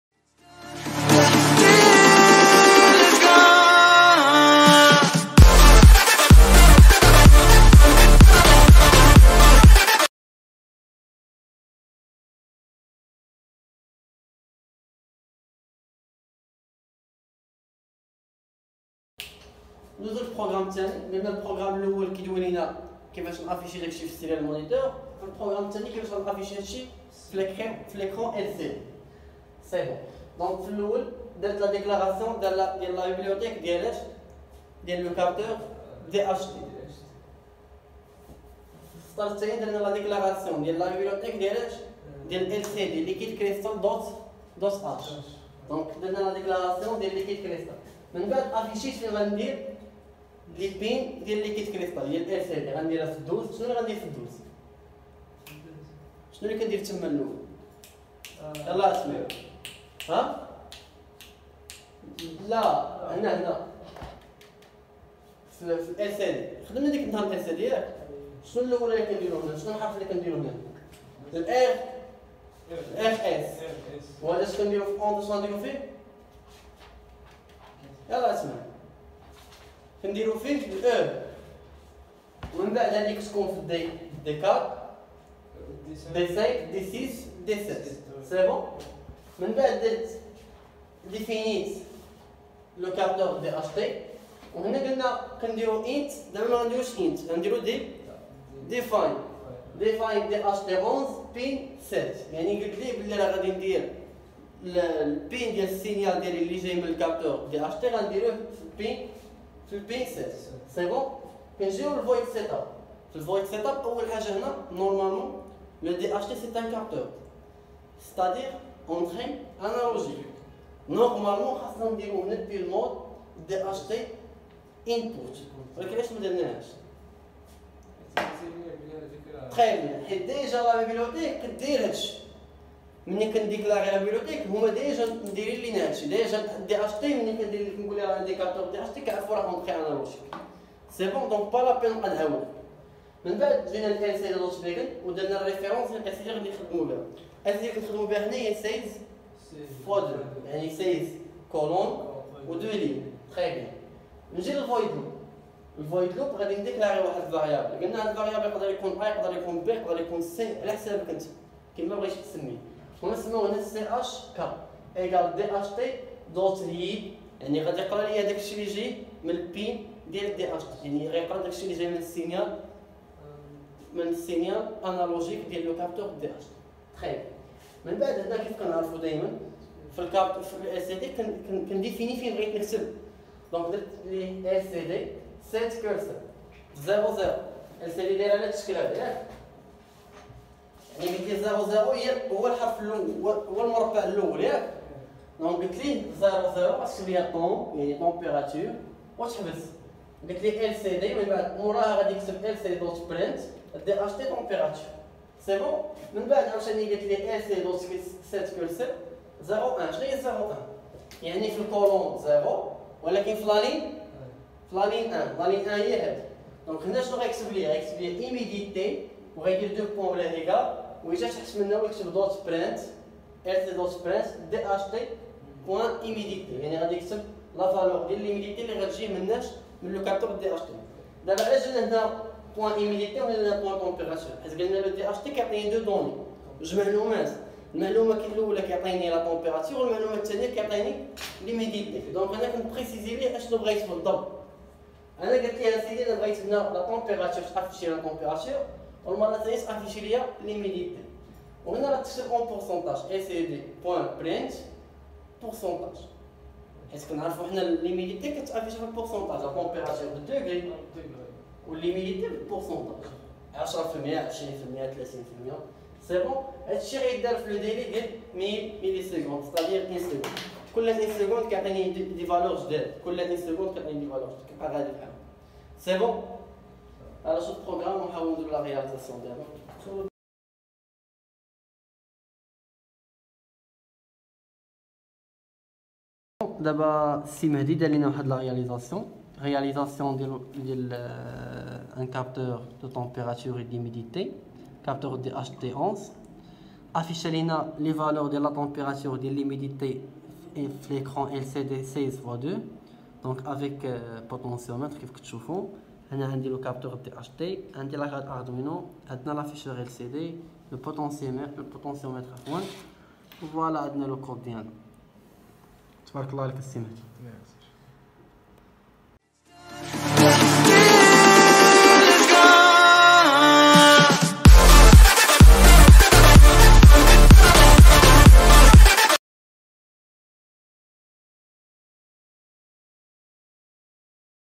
Formation Robotique TP N°8 : Afficher les mesures de Température et l'Humidité dans une écran LCD
⛔️⛔️ZINEDDINE_ACADEMY⚠️⚠️
🟡Formation Robotique
🟣TP N°8
🔴Afficher les mesures de la Température et l'Humidité avec capteur DHT dans une écran LCD 16x2

Dans cette vidéo je vous propose de réaliser un projet de la carte Arduino qui permet d'afficher les mesures de la Température et l'Humidité dans une écran LCD 16x2

Cette vidéo est une partie d'une séance d'une formation Robotique qui est destinée à toute personne souhaitant s'initier à la programmation des composantes électroniques pour la création du projet de robotique, de domotique, de borne interactive, automatisation de process

pour plus d'infos n'hésitez pas de nous contacter
⚪Compte Instagram :